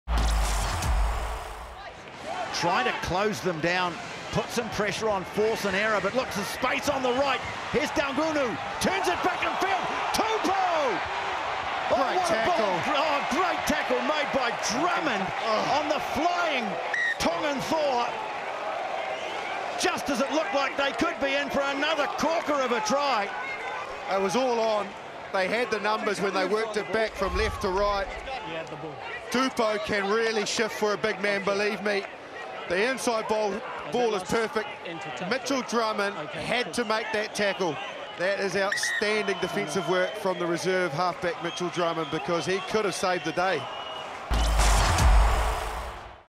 PLAY OF THE WEEK | Super Rugby 2020 Rd 6
Can Mitchell Drummond stop a Flying Tongan Thor in full flight? You bet he can. Did his heroic efforts save the Crusaders from an unlikely loss? Possibly.